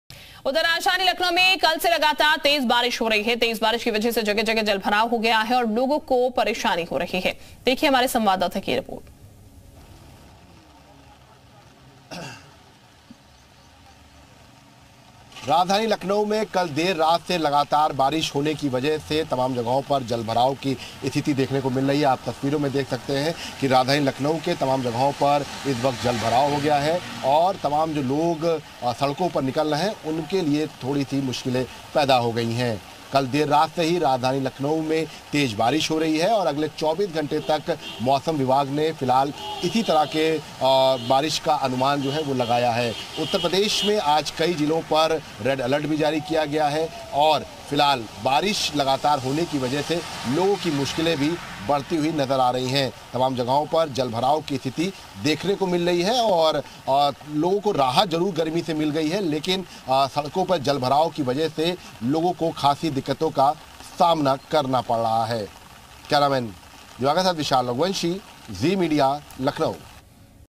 Lucknow Rainfall: राजधानी में कल से लगातार बारिश जारी | Heavy Rain | Water Logging | Monsoon
Lucknow: राजधानी में कल से लगातार बारिश जारी, भारी बारिश से जगह-जगह जलभराव, बारिश जोरदार..आफत की बौछार!